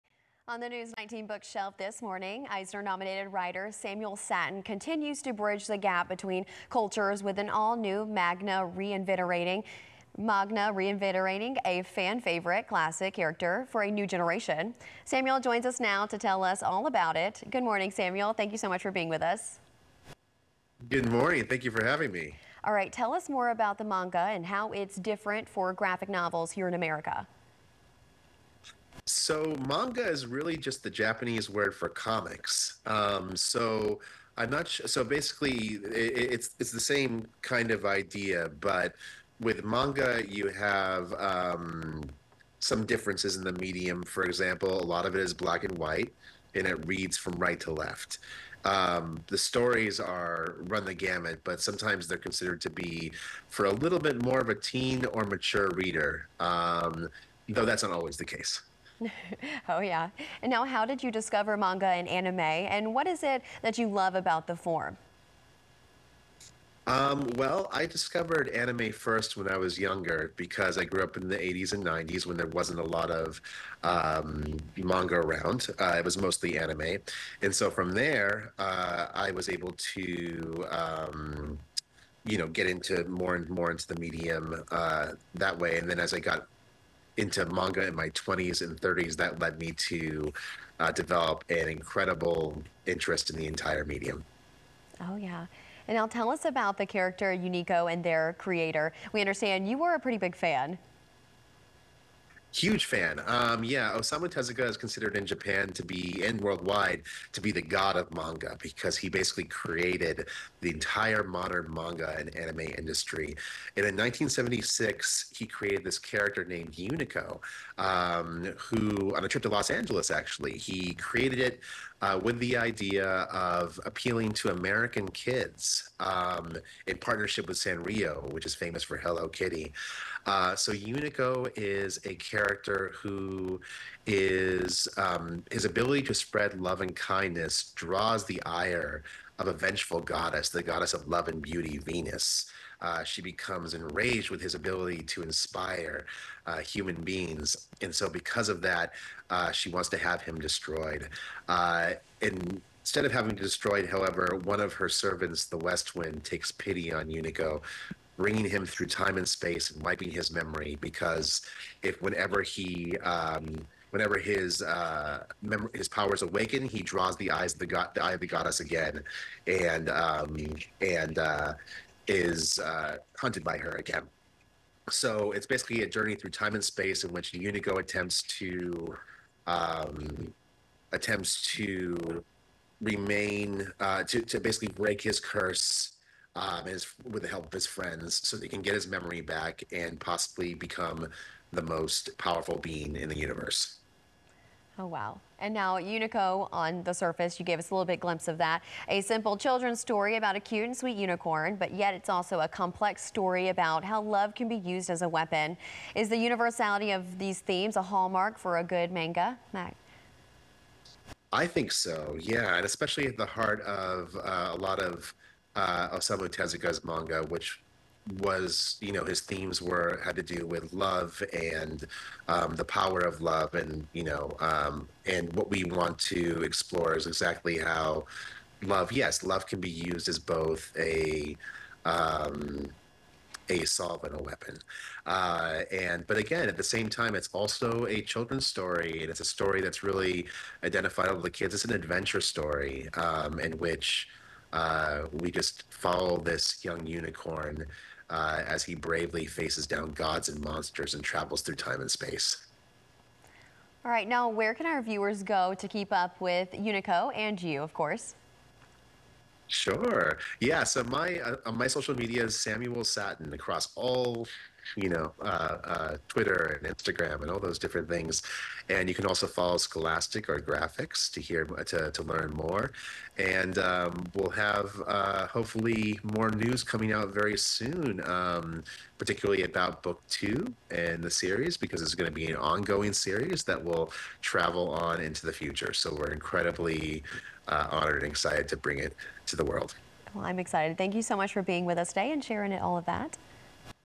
Manga For the Next Generation | Aug. 9, 2024 | News 19 at 9 a.m.
The Eisner-nominated writer Samuel Sattin (co-writer of The Essential Anime Guide)  and the Eisner Award-winning artist team Gurihiru (Superman Smashed the Klan, It’s Jeff web series) are reinventing “The Father of Manga” Osamu Tezuka’s character Unico for a new generation of readers, ages 8-12, grades 3-7.

Unico: Awakening is an all-new, 224 page manga with full color artwork. The project was originally introduced to fans with a smash-hit Kickstarter campaign in 2022. This title marks the first ever manga publication from Scholastic. The first volume of the highly anticipated, multi-volume publishing event will be released in 12 languages, reading left to right in the Western comics style to match the original manga.

The project has already won praise from critics and comics lovers, with The Mary Sue labeling it “One of the most anticipated books of 2024,” and author-cartoonist Dav Pilkey (Captain Underpants, Dog Man) dubbing Unicorn: Awakening “A spellbinding masterpiece.” Watch the official animated trailer for Unico: Awakening below and on Scholastic’s YouTube channel.